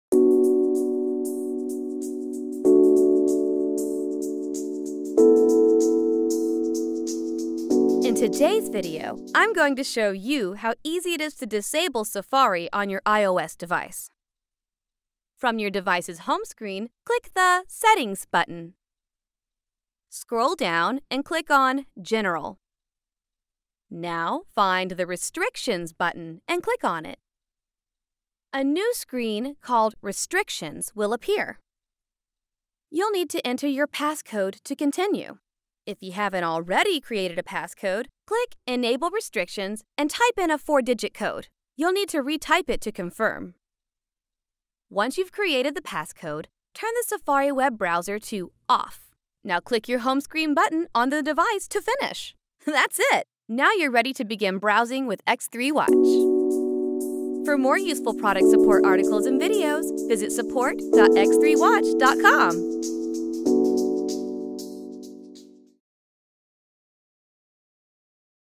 How to disable Safari on iOS
To use X3watch on your iPad, iPhone or iPod Touch, you'll need to disable Safari. This video will guide you through this process with easy step-by-step instructions.